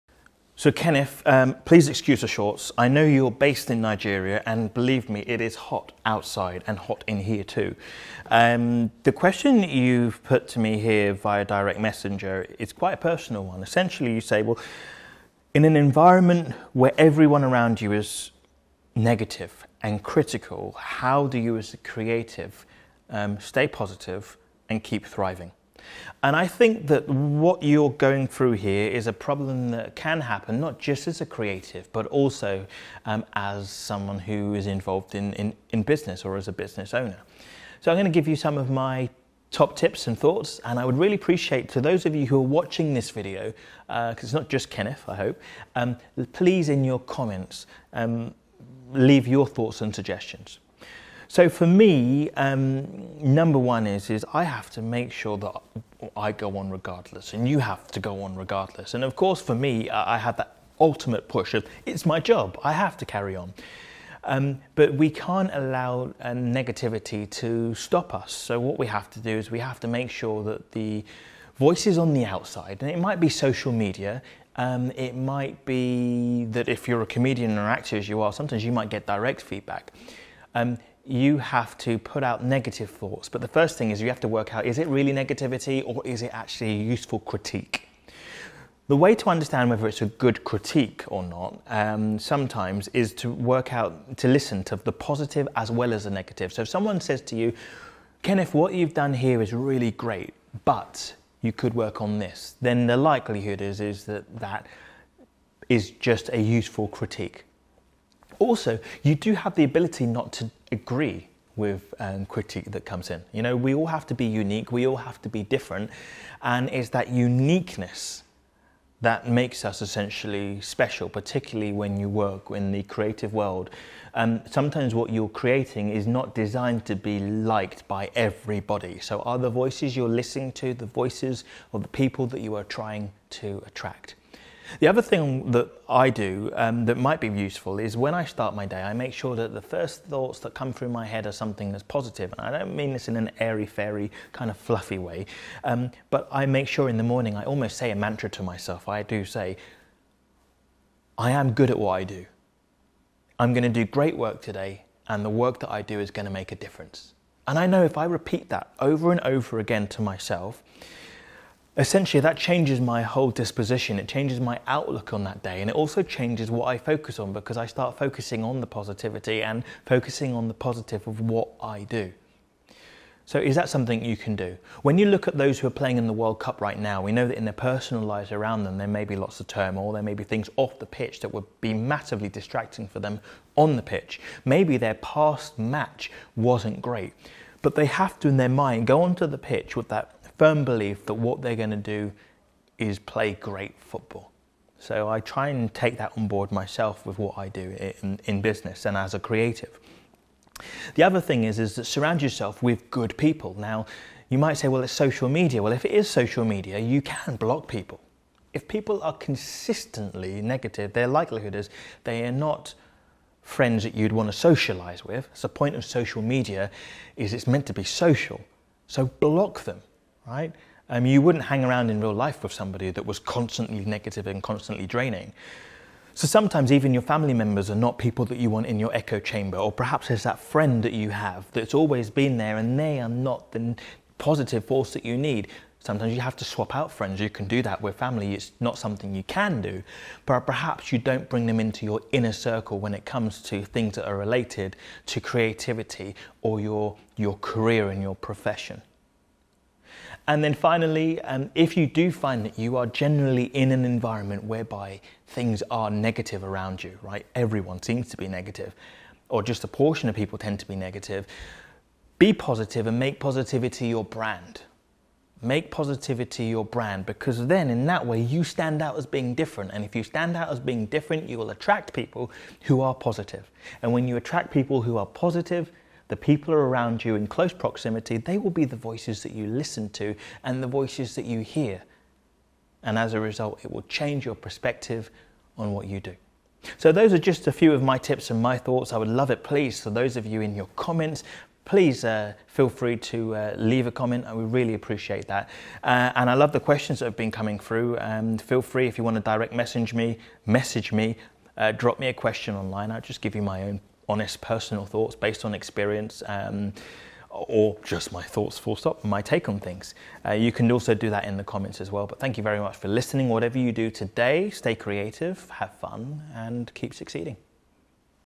Dealing with Negative People
How do you cope with negativity and critical people?  - This was the question sent to me by Kenneth, a young creative trying to start his journey in the industry as a comedian in front of the camera and also work in the industry behind the camera. But we can all be affected by this. What advice would you give?